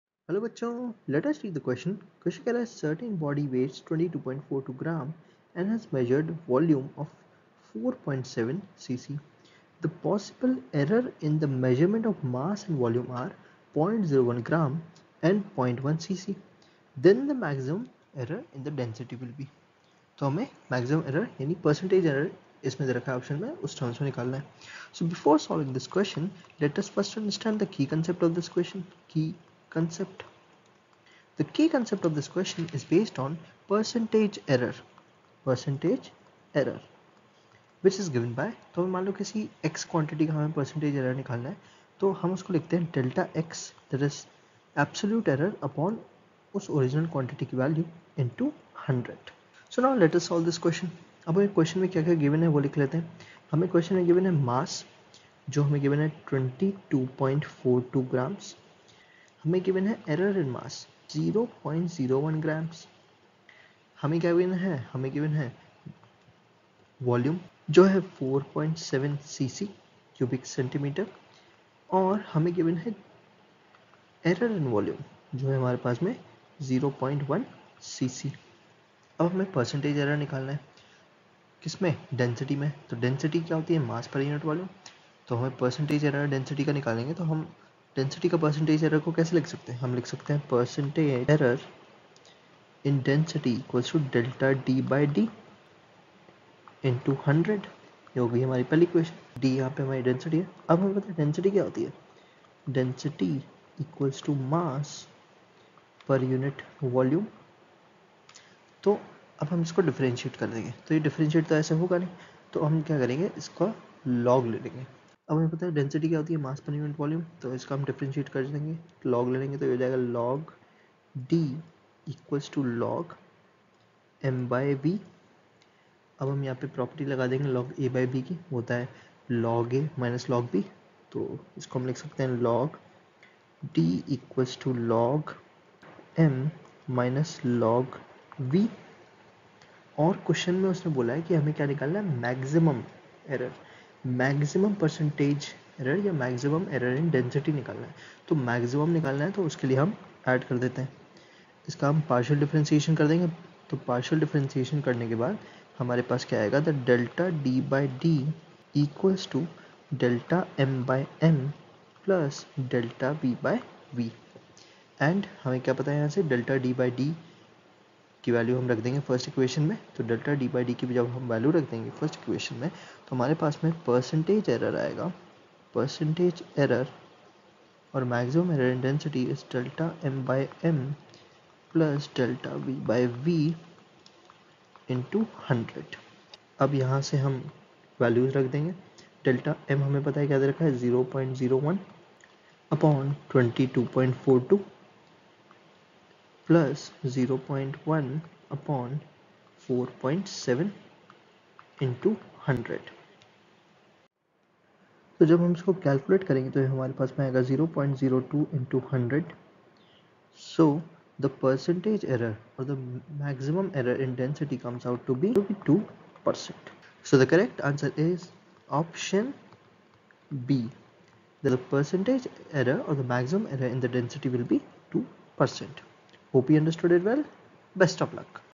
A certain body weighs22.42&#160;g and has a measured volume of 4.7cc. The possible, error in the....
A certain body weighs22.42&#160;g and has a measured volume of 4.7cc. The possible, error in the measurement of mass and volume are 0.01&#160;g and 0.1cc. Then maximum error in the density will be:MR*(199&#160;J) 📲PW App